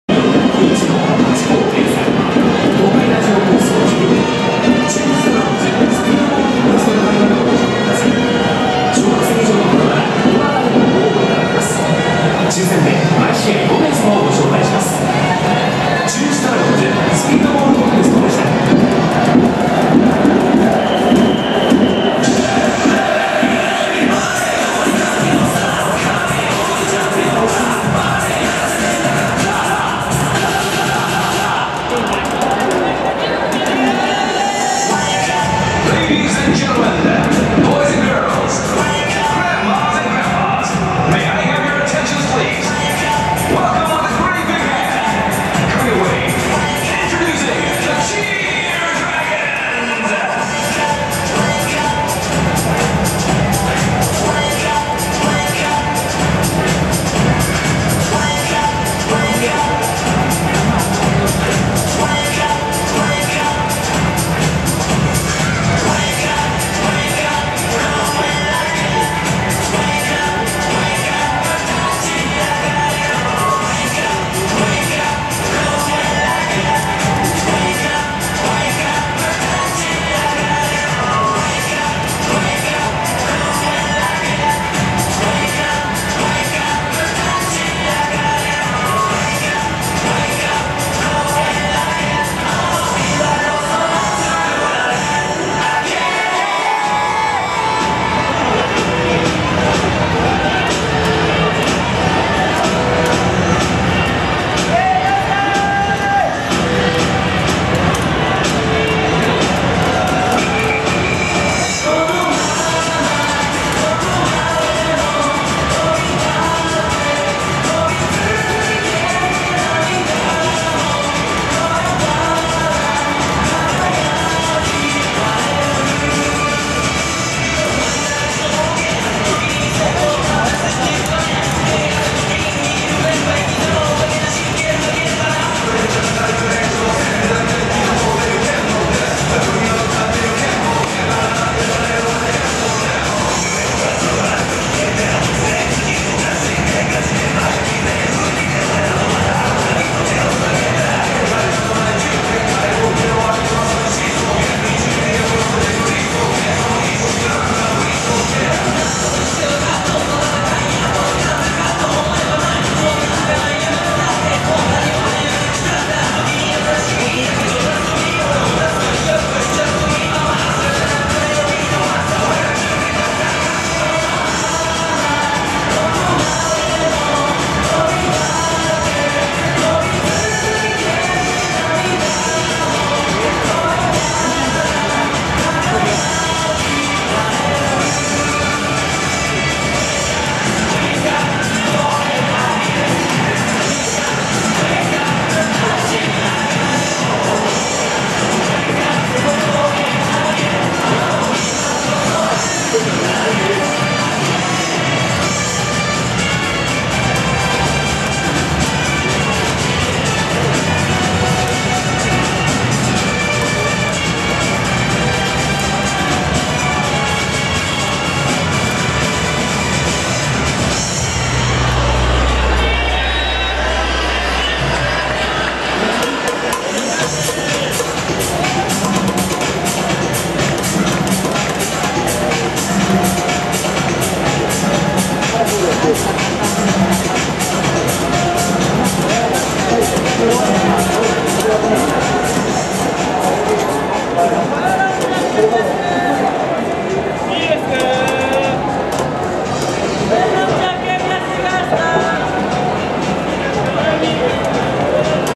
11/07/15 チアドラゴンズ2011 OPダンス スラィリーもいるよ
チアドラゴンズ2011とシャオロン、パオロン、ドアラによるオープニングダンス

7/15・16はスラィリーも参加です